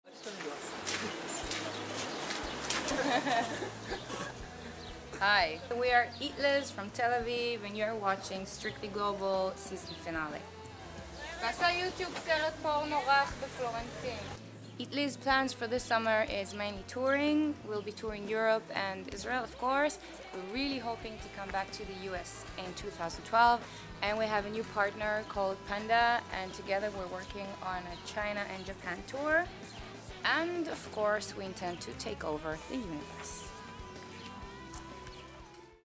Strictly Global Shoutout from Eatliz
Tel Aviv, Israel based rock band Eatliz send a shout-out and upcoming news on Strictly Global's series finale.

Footage courtesy of Eatliz.
Edited by Dawn Reed